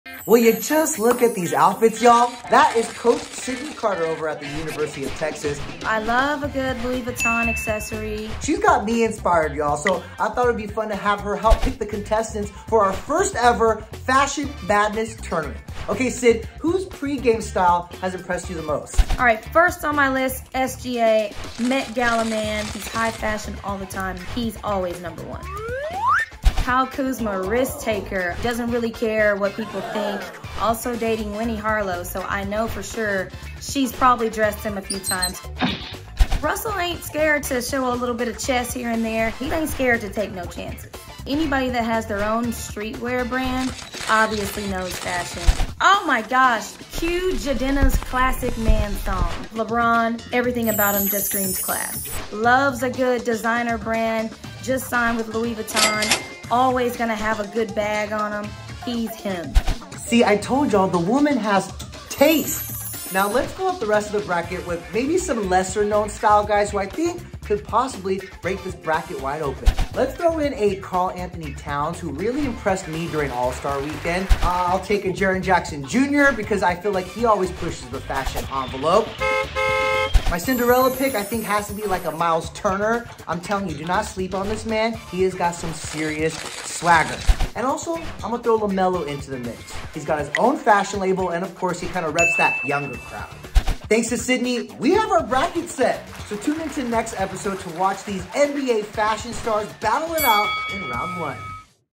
Fashion Face Off: Fashion Madness Round 1
Social media video edited for Bally Sports in which we created a "Fashion Madness" tournament in correlation to March Madness.